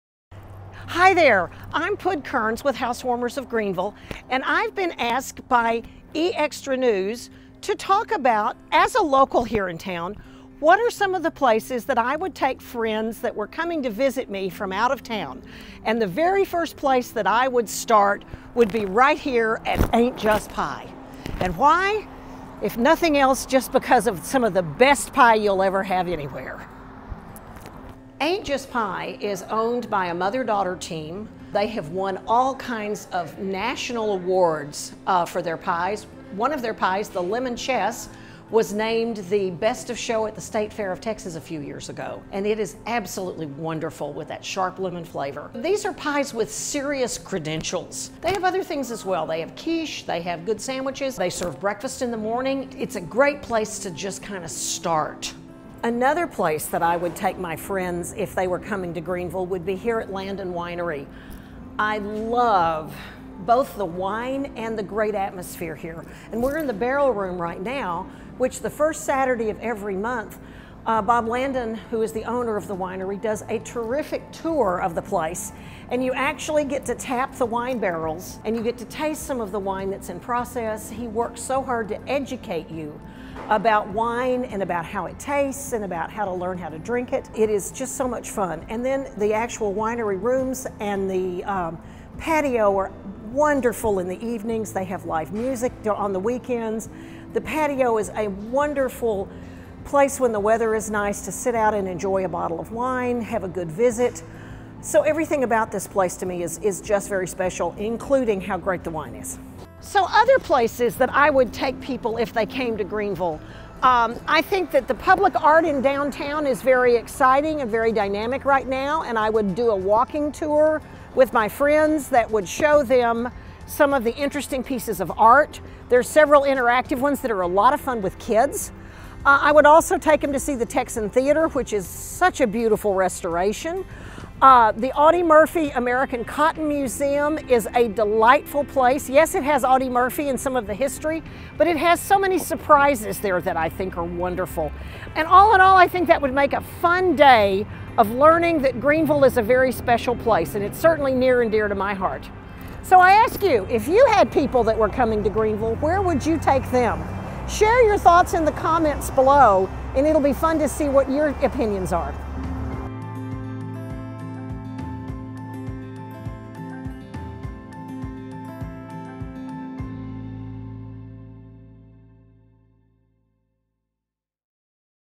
Pud Kearns || Ask a Local: Greenville, Texas
As the first month of the new year comes to a close, we’ve asked local Greenville resident, Pud Kearns, where she would take family or friends visiting beautiful Greenville, Texas.

Tune in next month when we ASKALOCAL more about this great city!